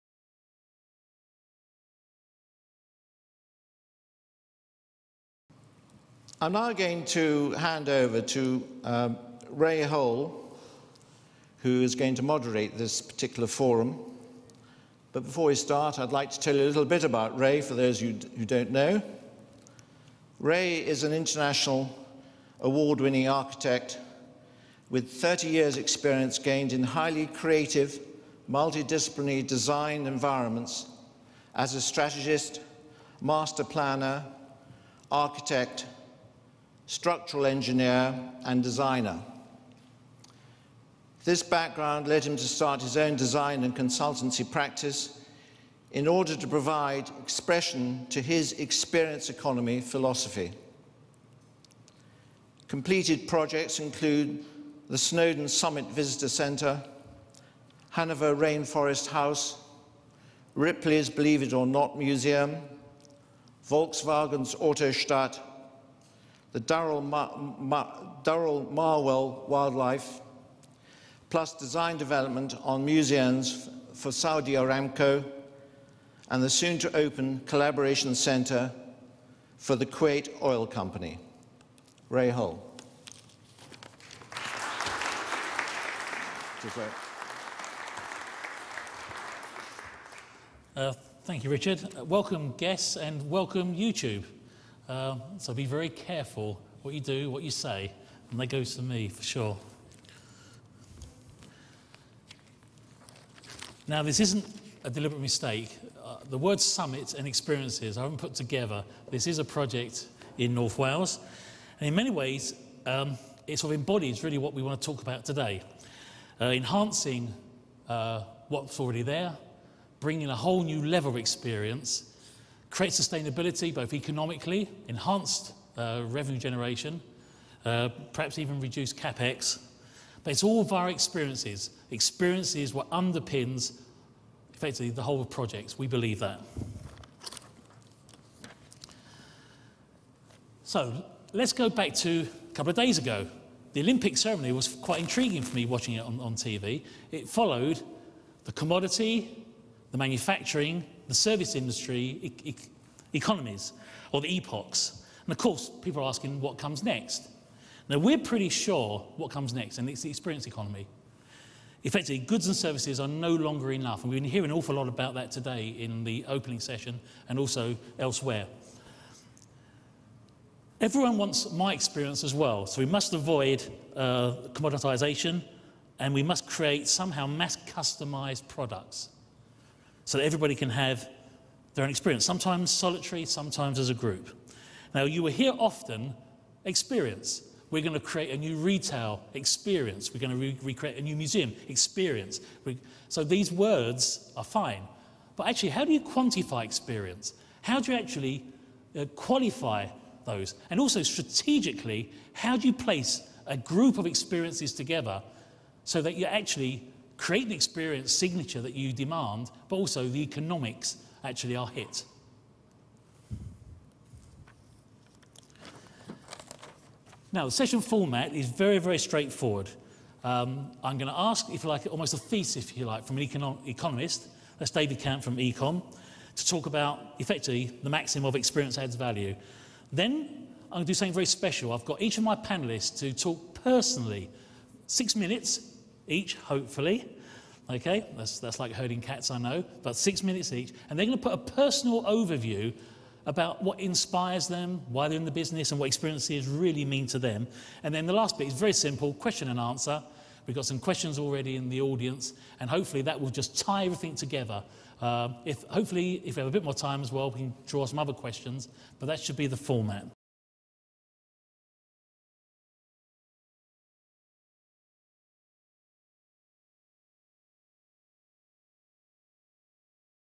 Creative Services Summit - Ray Hole, Managing Director, Ray Hole Architects
Ray Hole, Managing Director, Ray Hole Architects speaking at the Creative Services Summit during the British Business Embassy on 30 July 2012.